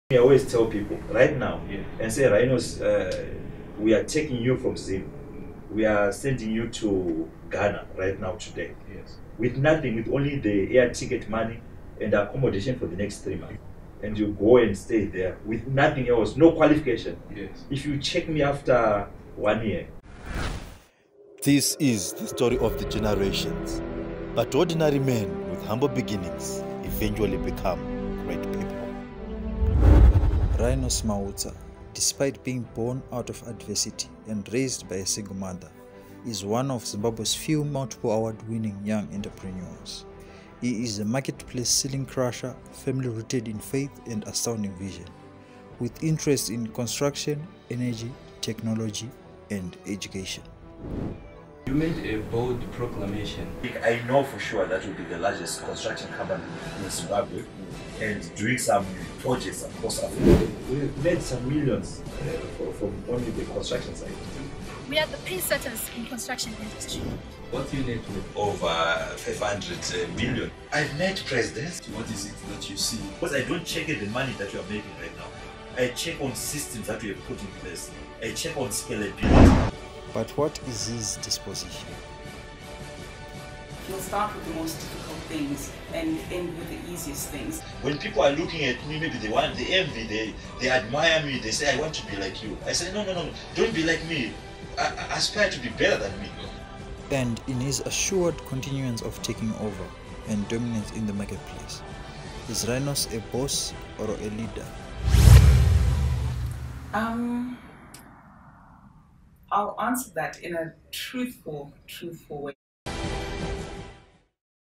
Rinos Mautsa interview trailer
Full interview is coming soon in frame. Rinos Mautsa and one representative from Emperors of success team